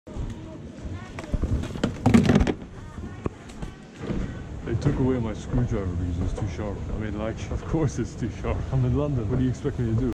British authorities take away my screwdriver
UK left the EU and now British airport security is spawn trapping all connections into London